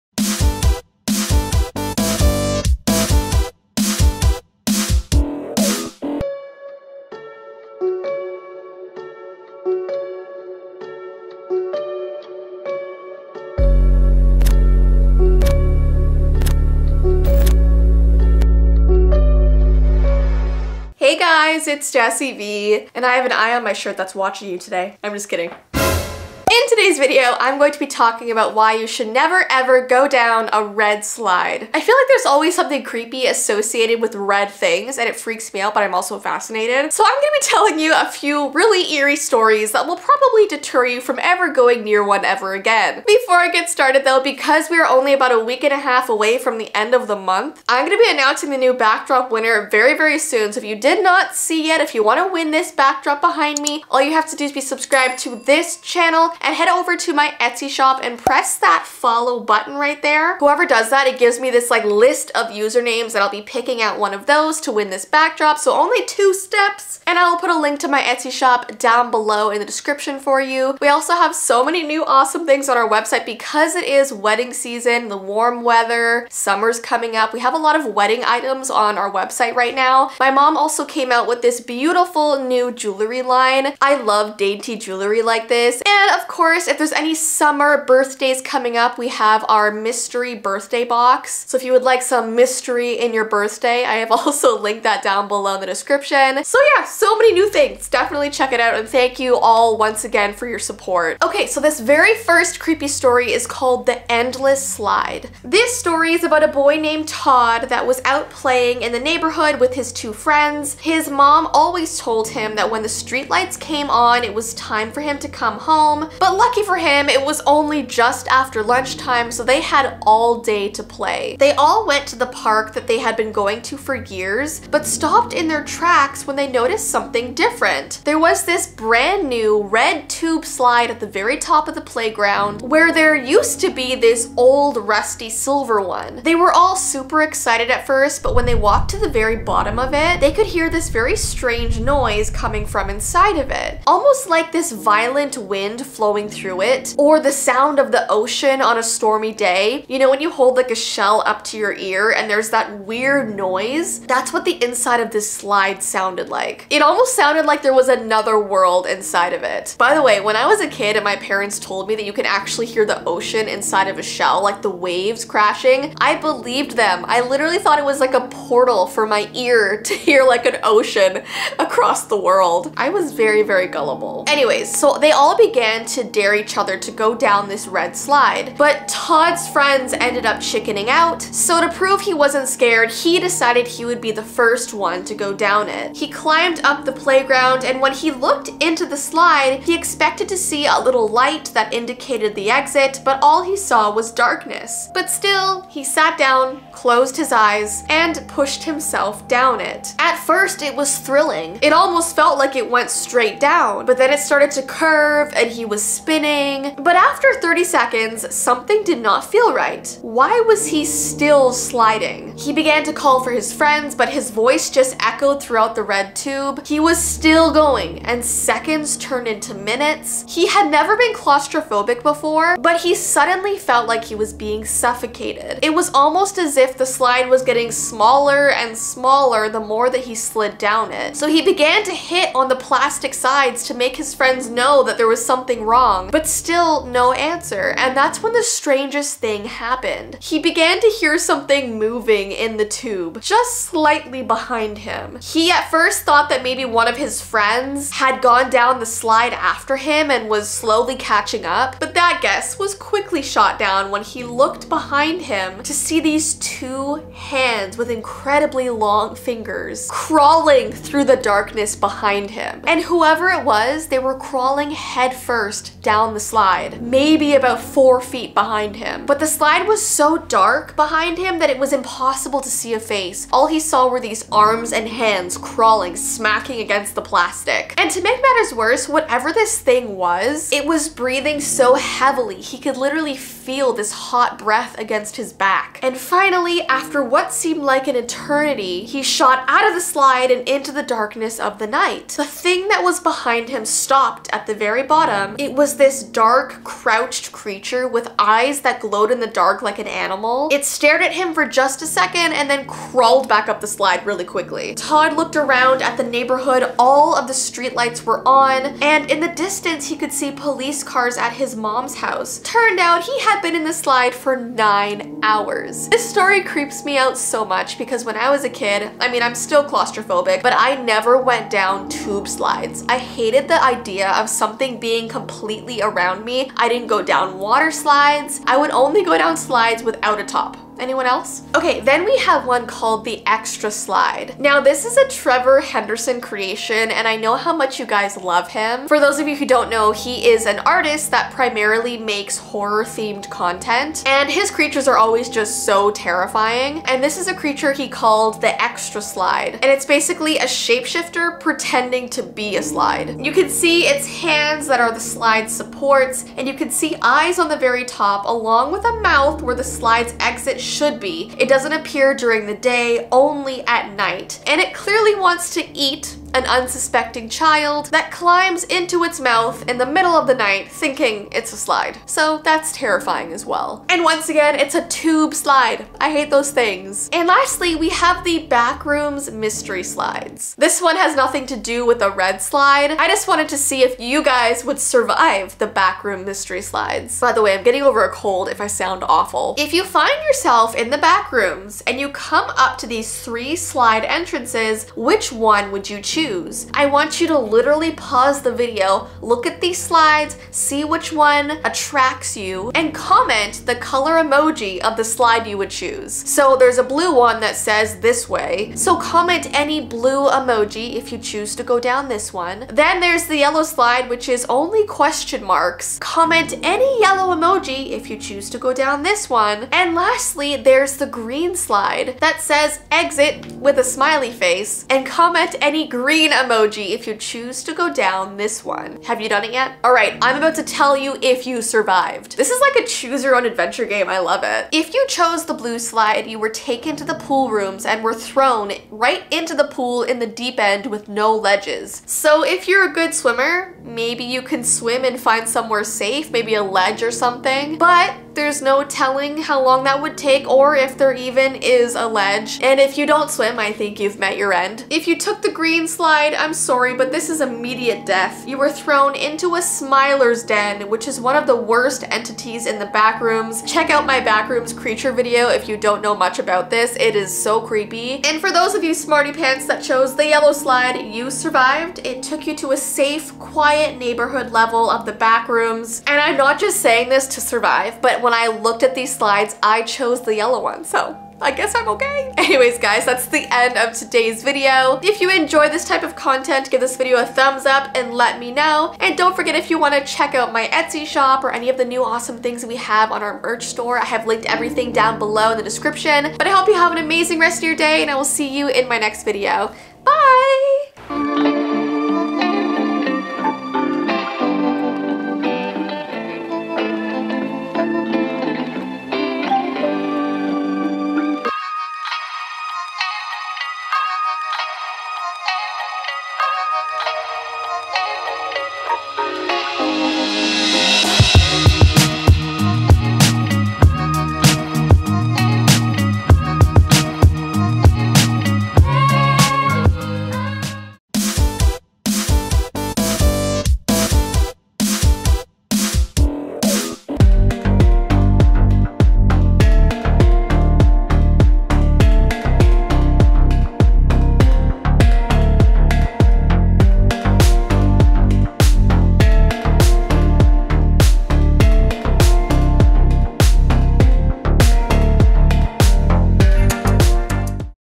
Why You Should NEVER Go Down A Red Slide...
In today's video I'm talking about why you should never go down a red slide...

My Etsy Shop

Jessii Vee
# 57012
Brampton, Ontario
Canada
L6Z 0E7